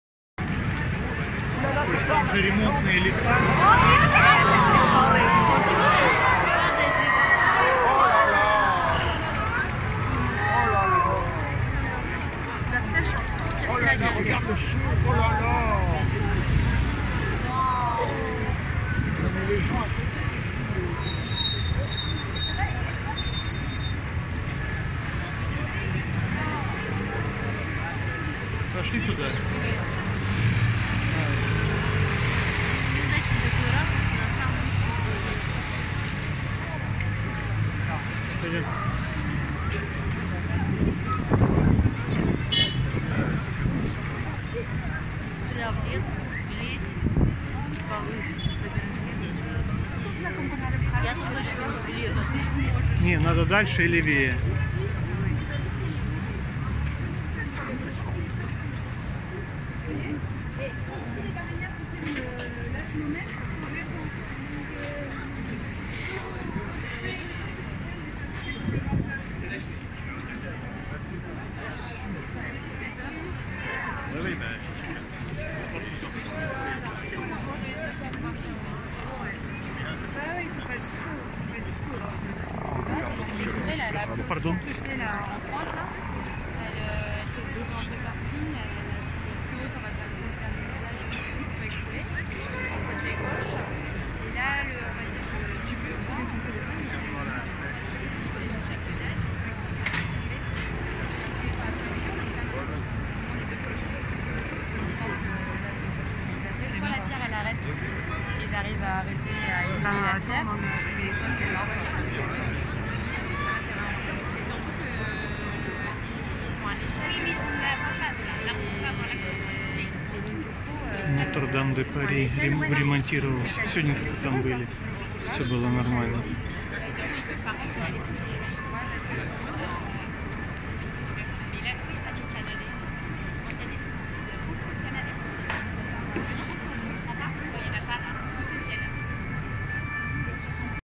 В Париже горит Нотр-Дам де Пари
Пламя охватило собор 15 апреля в 19:50 по московскому времени. Подробности